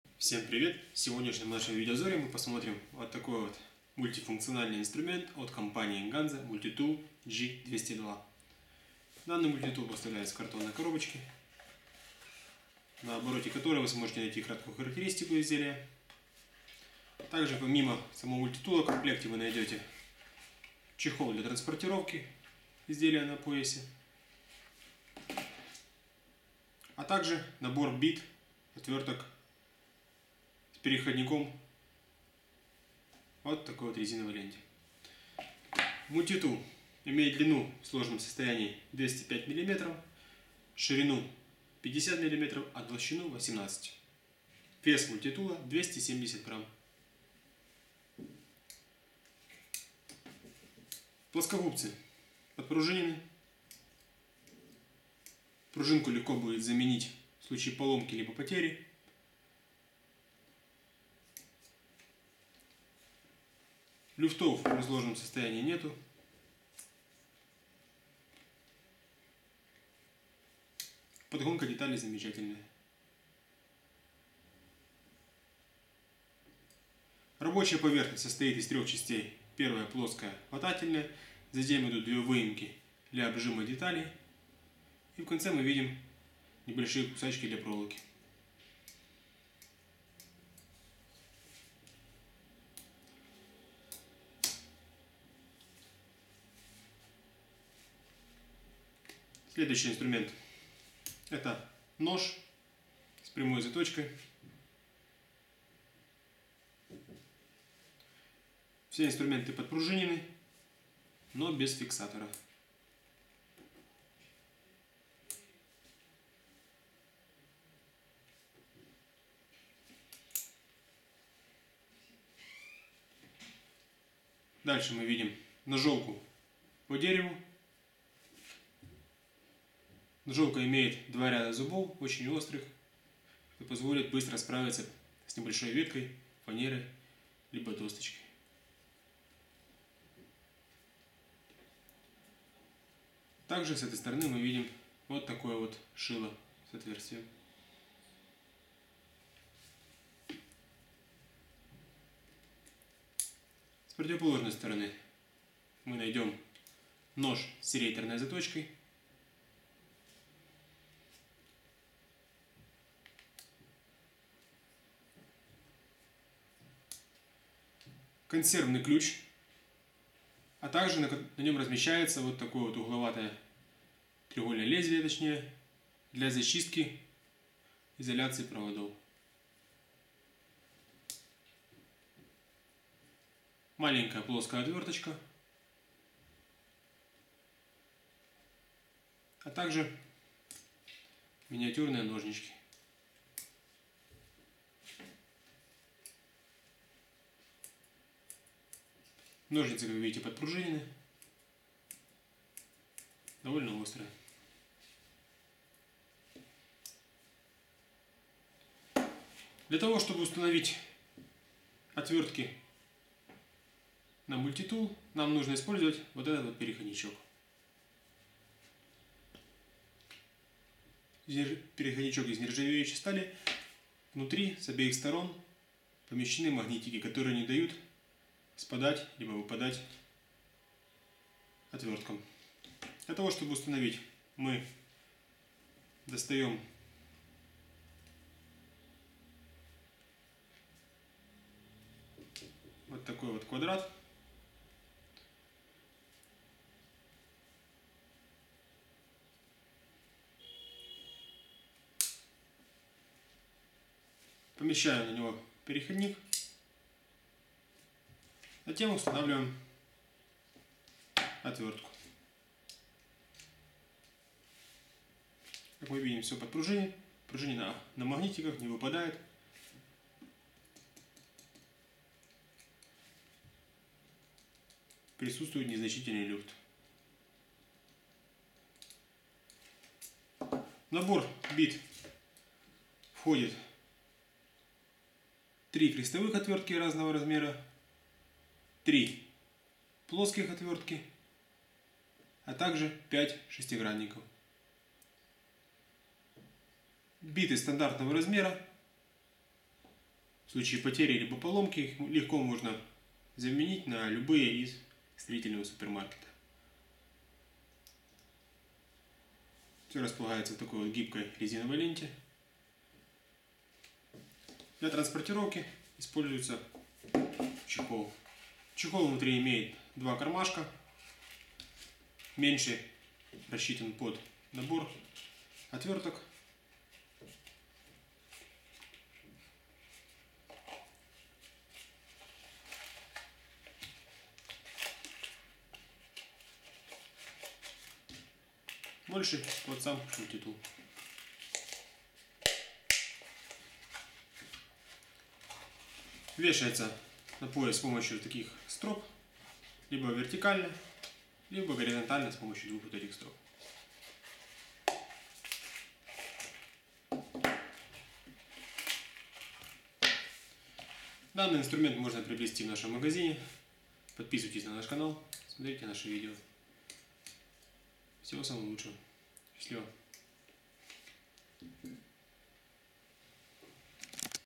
Мультитул ганзо Ganzo G202 multi-tool
Мультитул ганзо Ganzo G202 multi-tool включается в себя 22 разных инструментов, из которых 12 отверток. Незаменимый девайс в любых ситуациях.